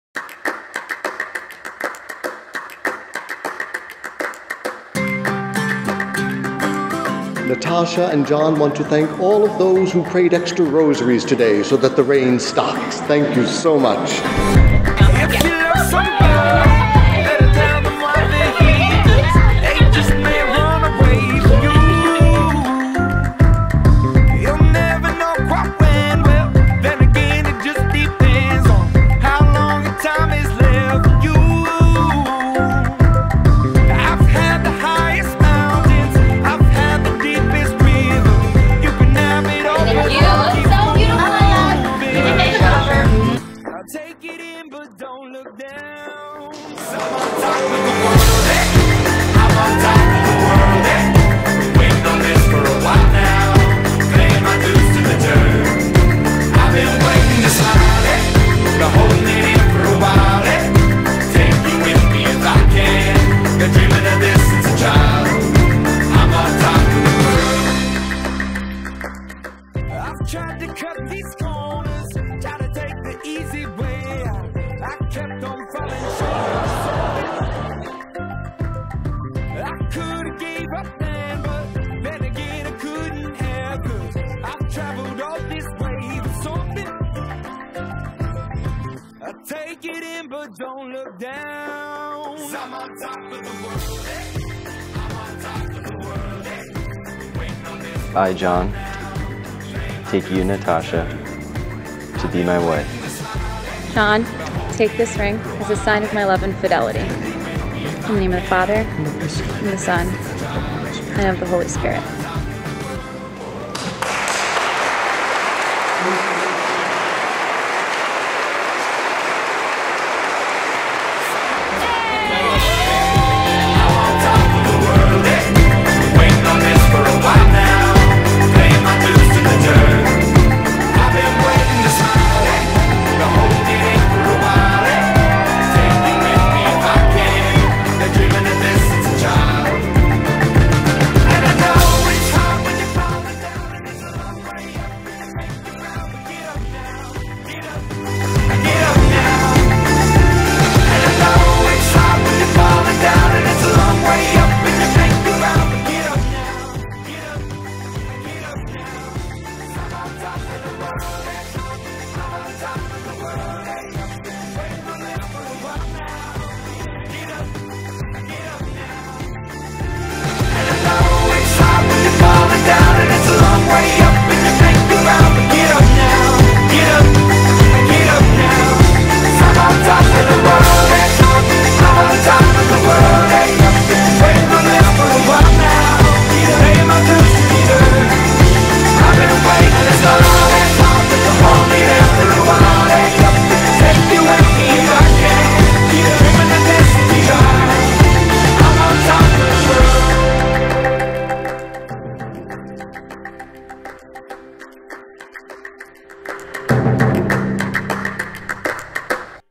Natasha and John | The Central Park Boathouse Wedding
Natasha and John's wedding video, filmed by eMotion Pictures Wedding Films at the Central Park Boathouse in New York City.